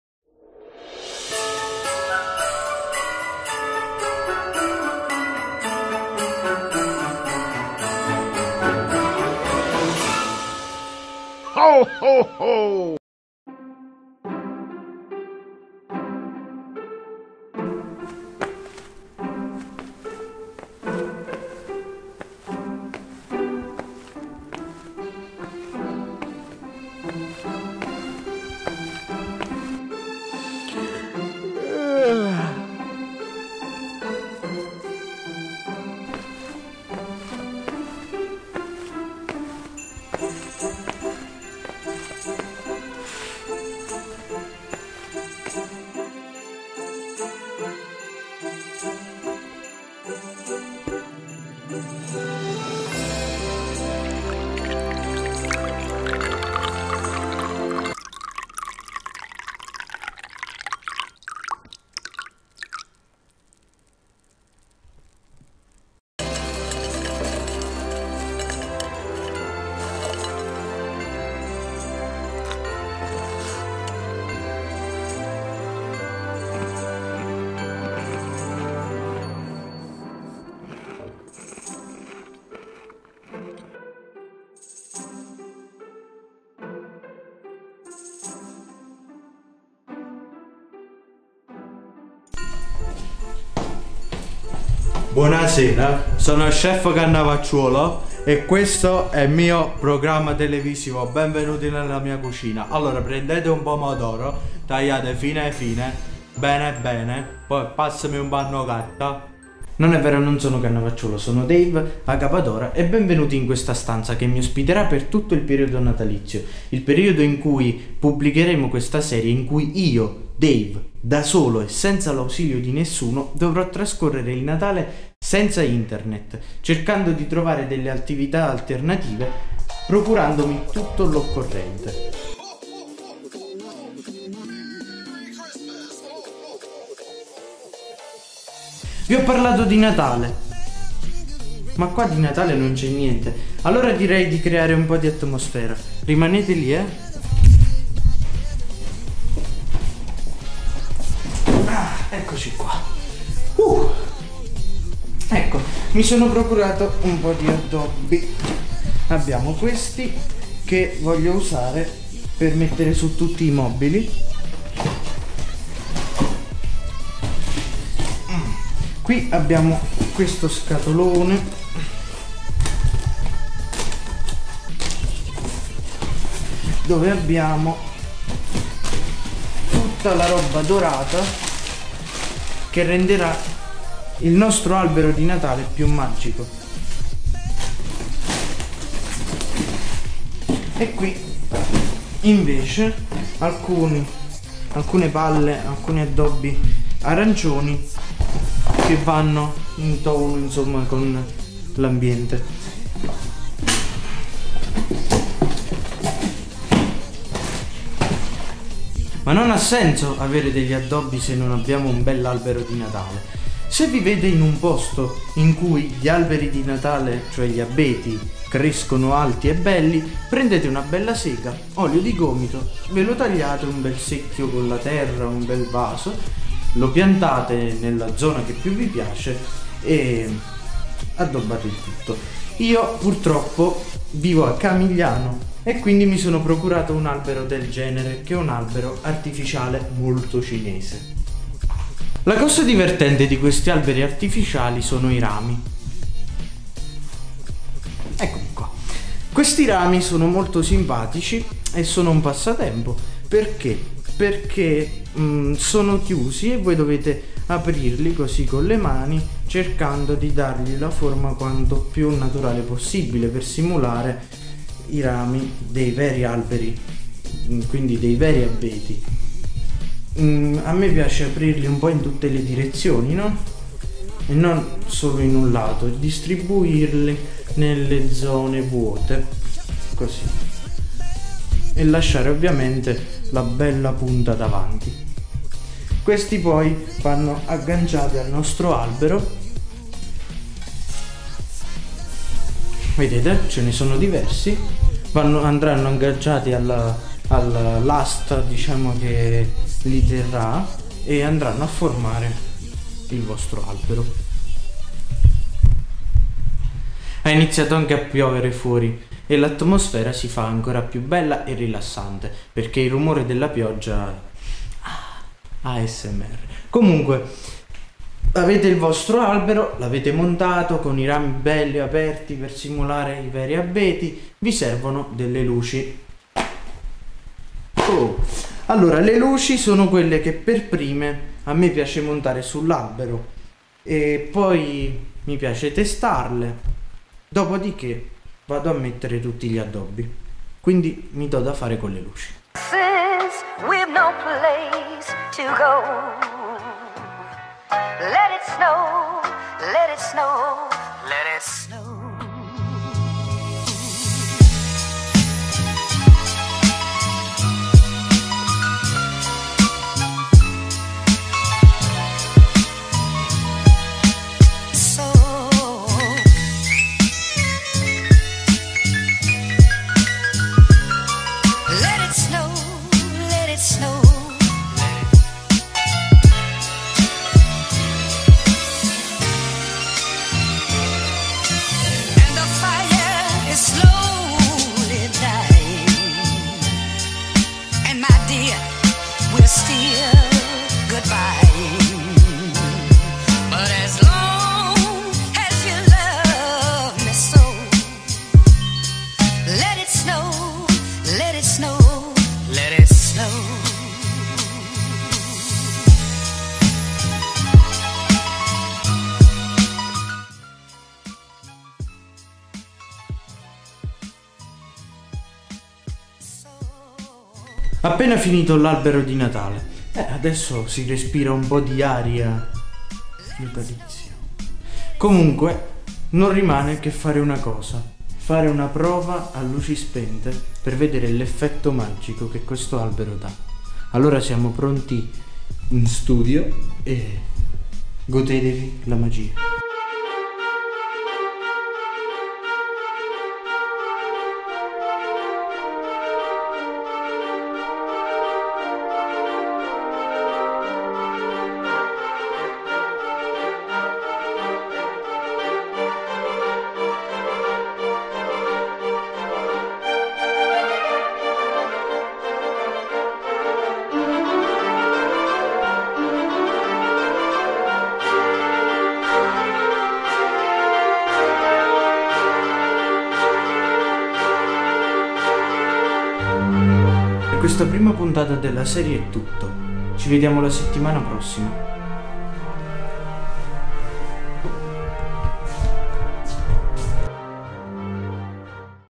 COME FARE L'ALBERO DI NATALE 2018
Come fare l'albero di Natale?
Ce lo spiega Dave!